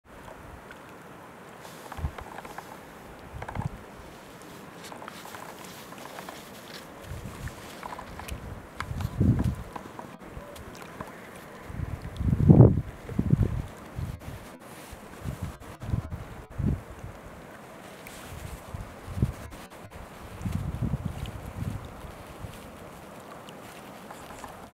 Sierra Garter Snake
Garter Snake swimming towards the camera at Ediza Lake